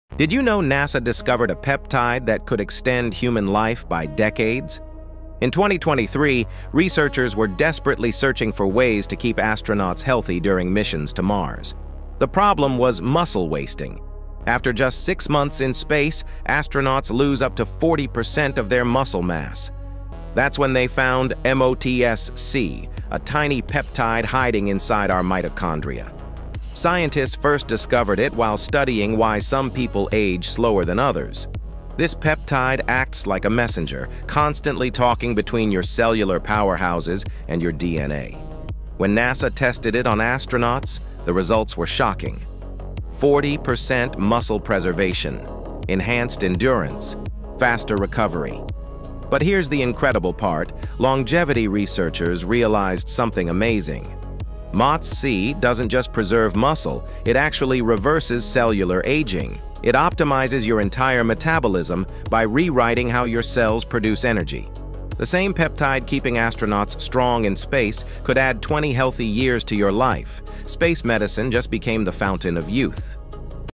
NASA Found a Peptide That Could Add 20 Years to Your Life
NASA peptide discovery could extend life by decades 🚀